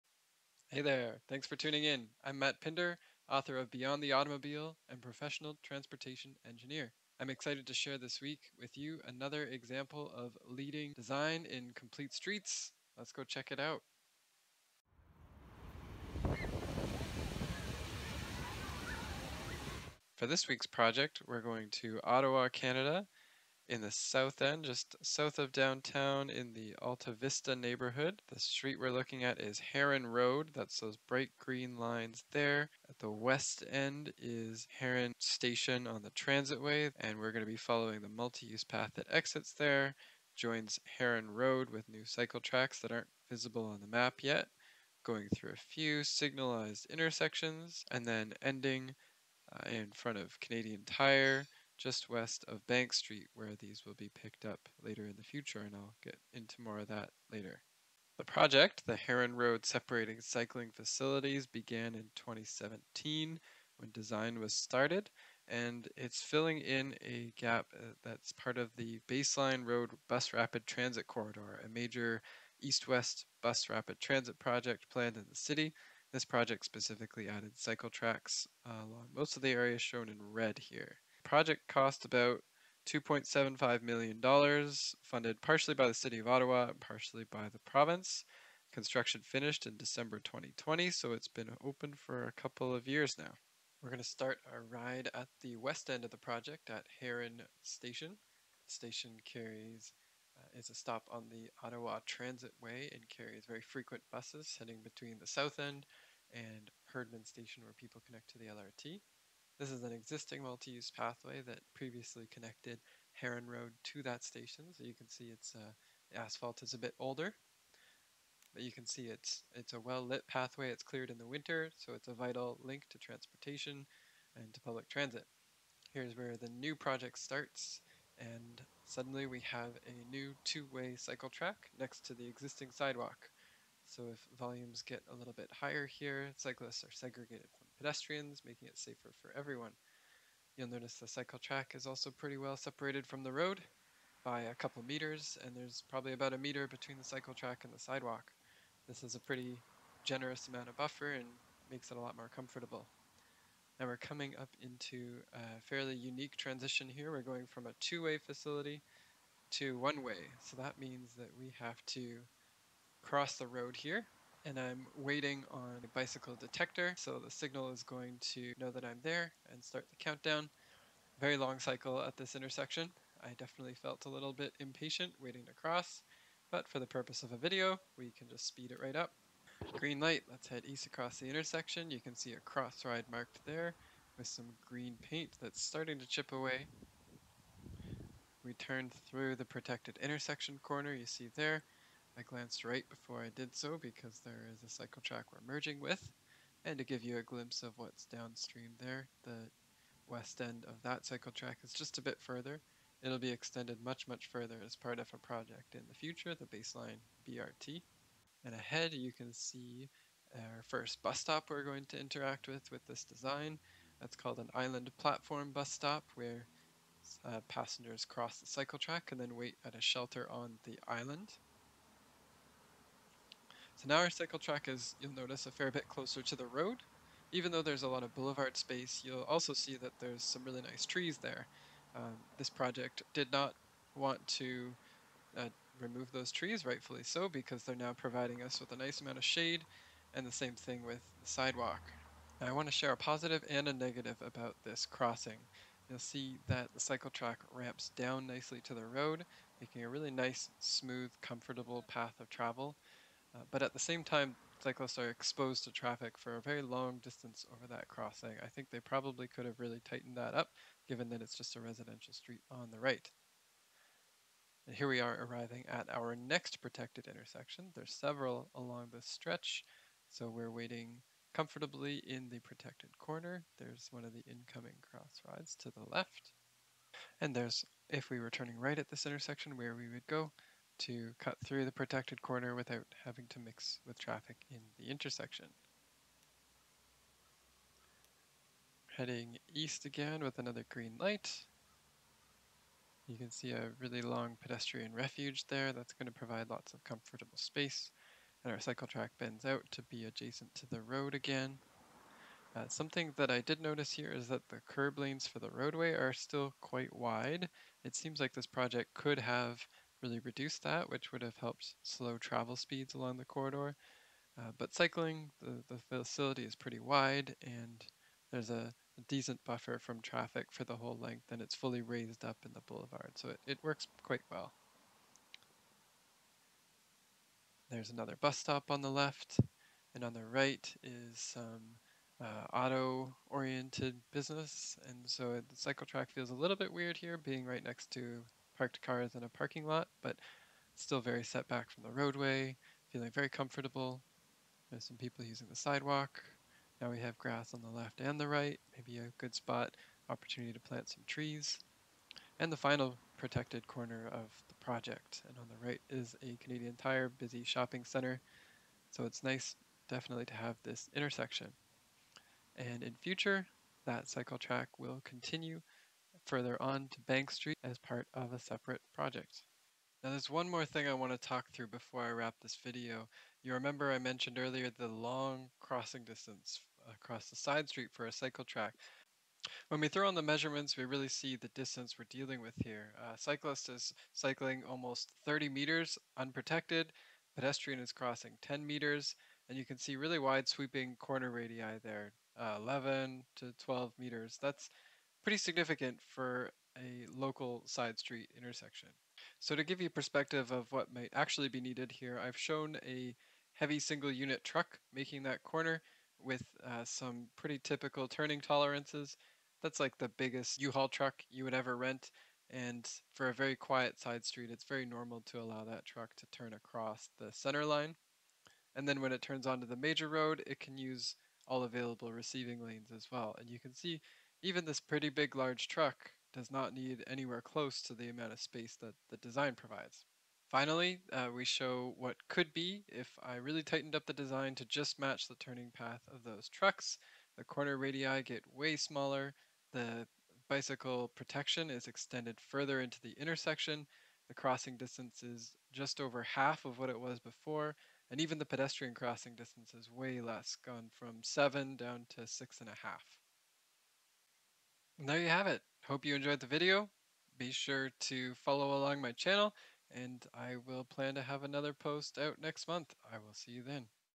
Riding Easy on Ottawa's Heron Road Cycle Tracks
Commentary from an engineer who designs this stuff!

More of a "showcase" project than a practical link at this point, Ottawa's Heron Road Cycle Tracks show how cycling on a busy arterial road can be made much less stressful with physical separation and protected intersections. Once a couple of connecting projects are completed, this will form part of an east-west spine across the city. For now, at least you can ride from the bus station to Canadian Tire!

00:00 Intro and about me
00:20 About the project
01:33 Riding the route
06:10 Idea for improvement: tighten the corners
07:34 Wrap up